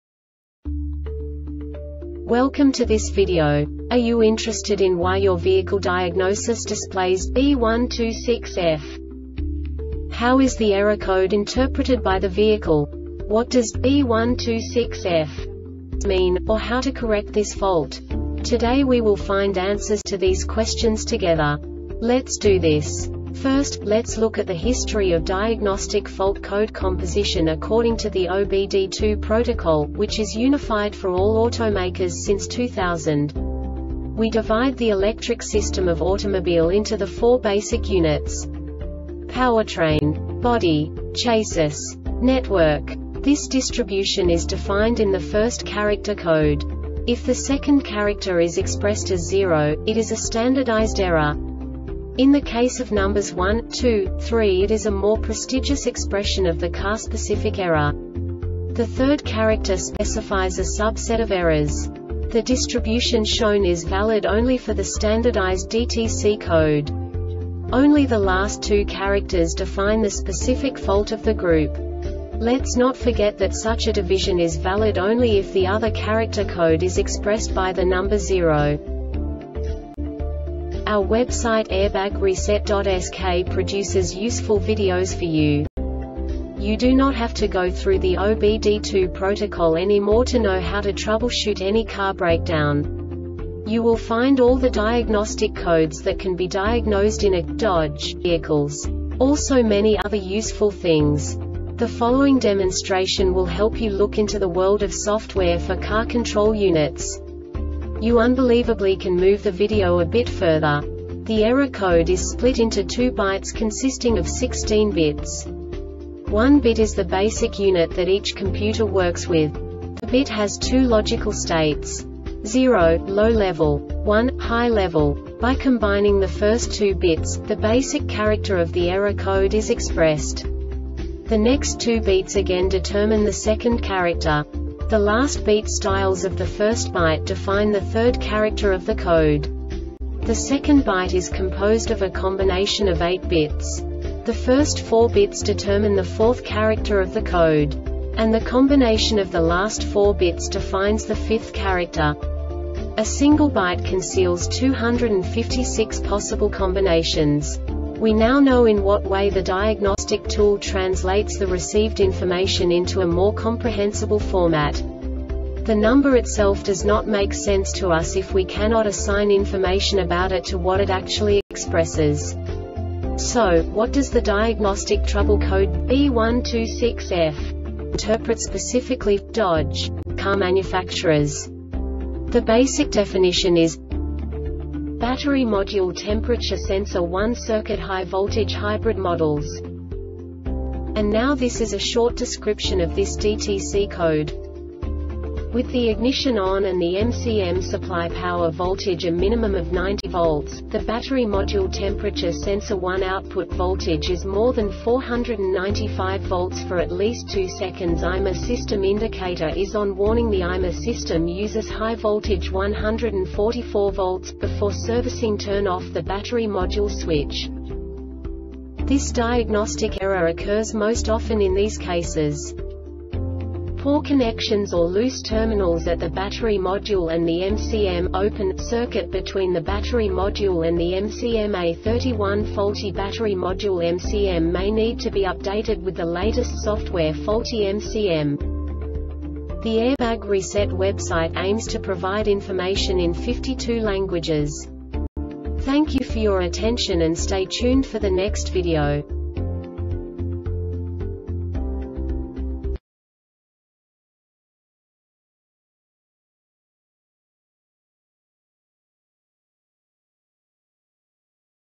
DTC Dodge B126F Short Explanation
Contents:
0:21 Basic DTC analysis according to OBD2 protocol standard.
1:48 Insight into programming
2:58 A diagnostically understandable description of the Diagnostic Trouble Code
3:05 Basic error definition

Make
Dodge
Code
B126F
Definition
Battery Module Temperature Sensor 1 Circuit High Voltage HYBRID MODELS
Description
With the ignition ON and the MCM supply-power voltage a minimum of 90 V,  the battery module temperature sensor 1 output voltage is more than 495 V for at least 2 seconds  IMA system indicator is ON WARNING The IMA system uses high voltage 144 V,  before servicing turn OFF the battery module switch
Cause
 Poor connections or loose terminals at the battery module and the MCM "Open" circuit between the battery module and the MCM A31 Faulty battery module MCM may need to be updated with the latest software Faulty MCM